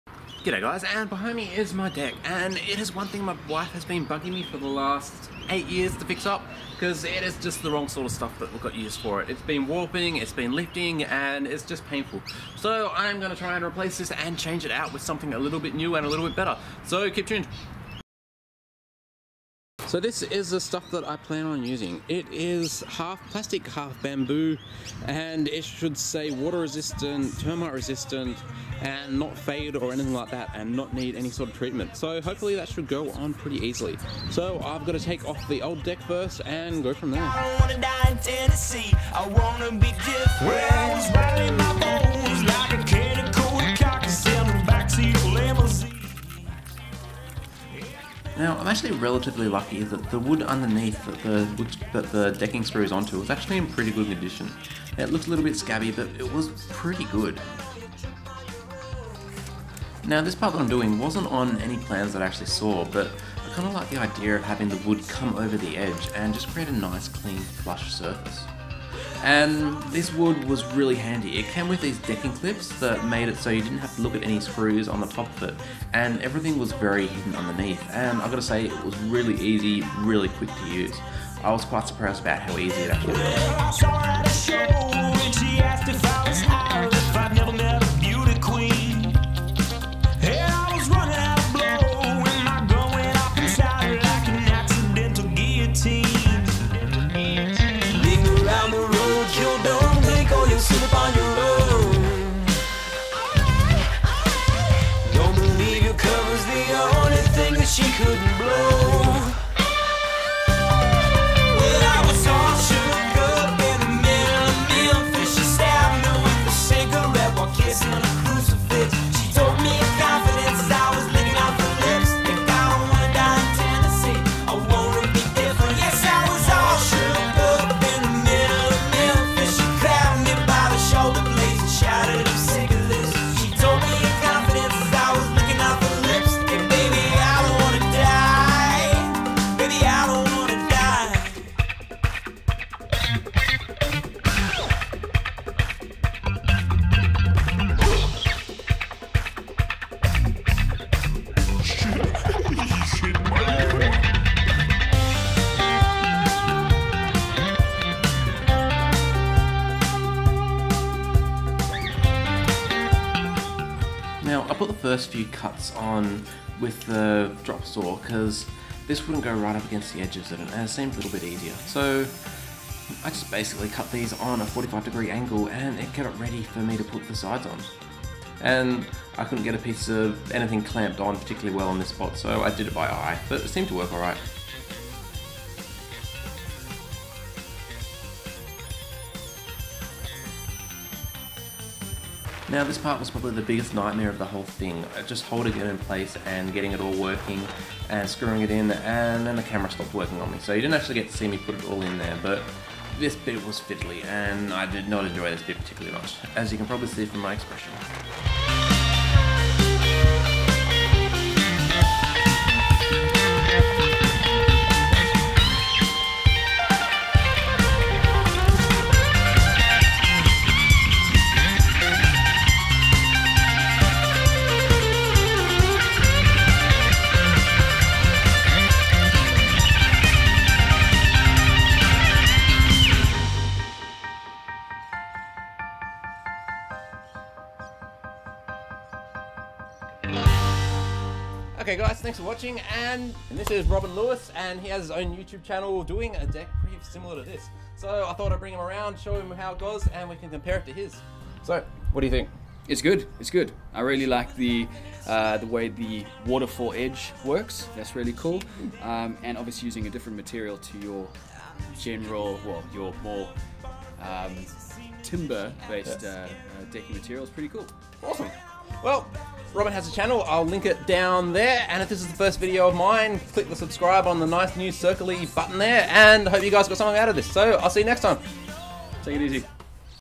I Resurfaced my Deck - Plastic/Bamboo Composite - Ekodeck
My Deck was looking very sad, we're rubbish at oiling/painting the existing hard wood, and it deteriorated quickly.  So I went through the process of changing it.

Any questions, please let me know.

That's Robin Lewis in the end, check out his channel here, he's got some great content: